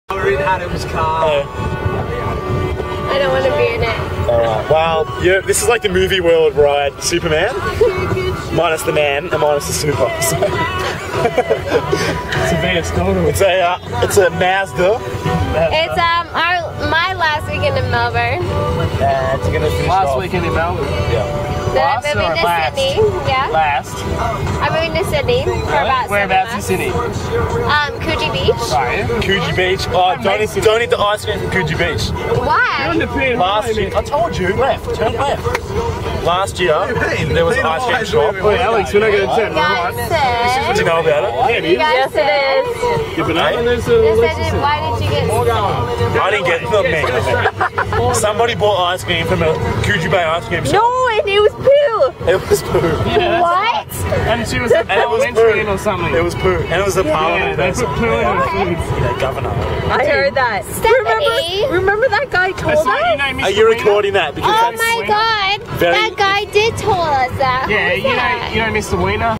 A Random Car Ride
The random conversations that occur when guys offer to drive you to the bar